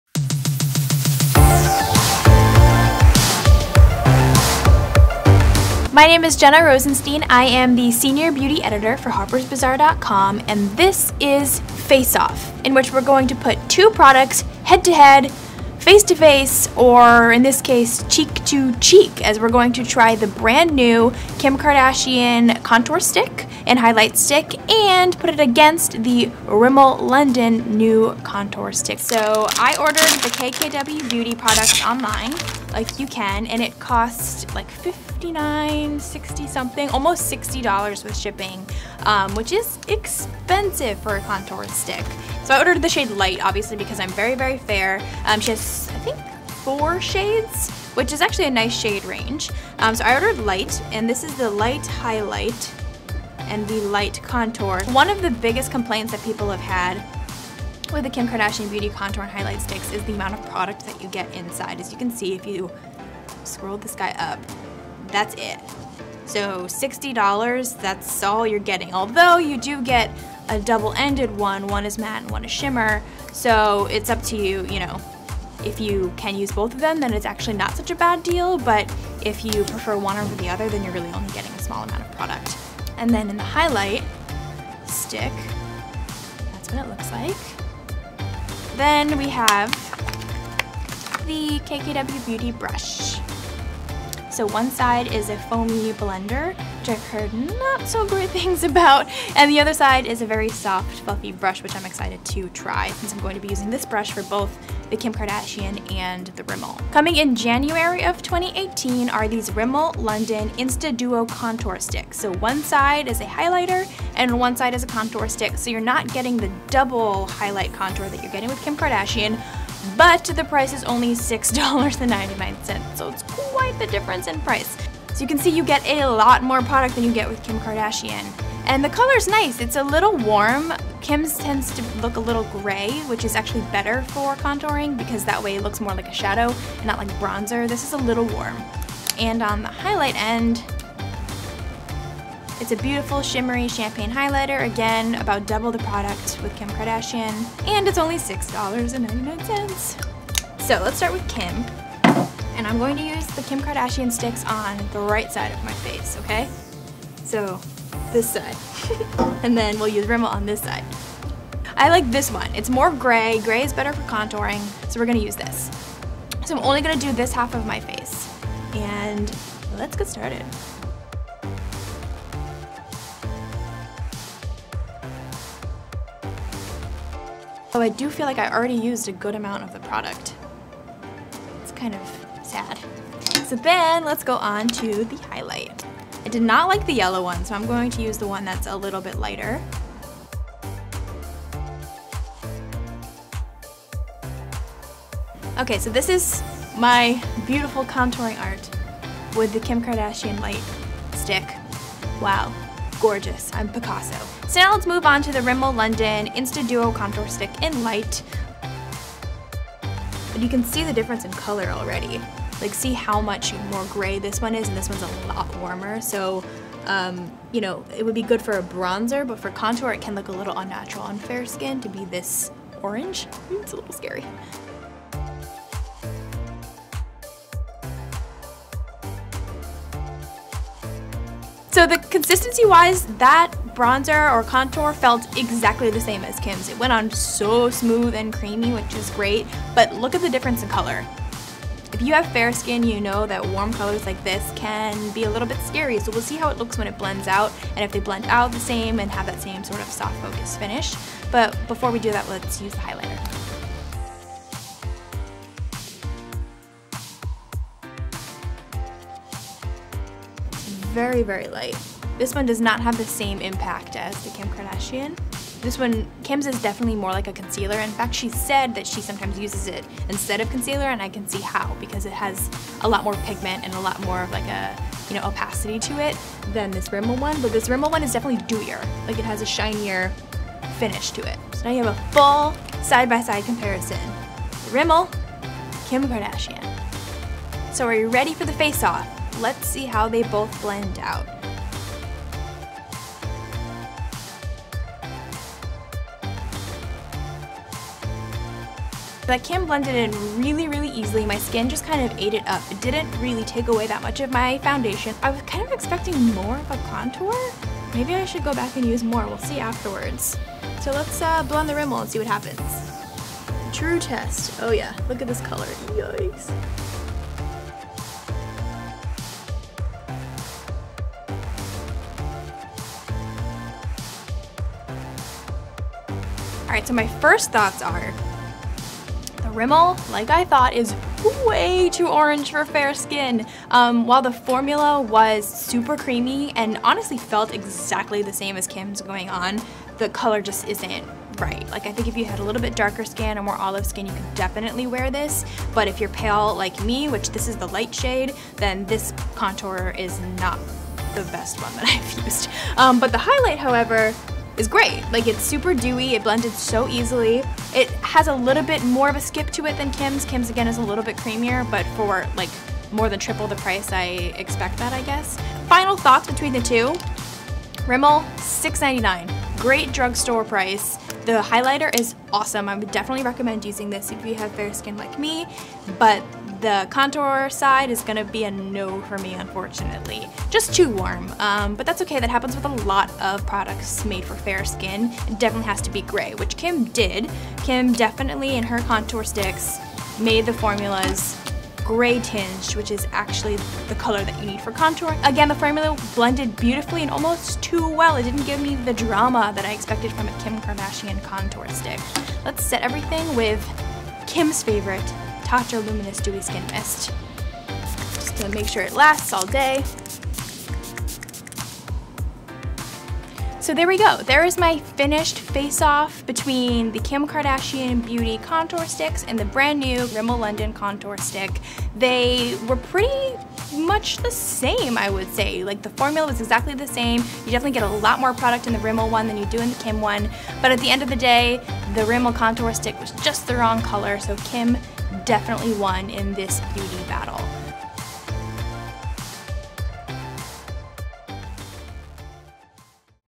Testing $60 KKW Beauty vs. $7 Drugstore Contour Sticks | Harper's BAZAAR
We're putting KKW Beauty's Créme Contour & Highlight Kit to the test against Rimmel London's new Insta-Duo Contour Stick.

Which do you think looks best?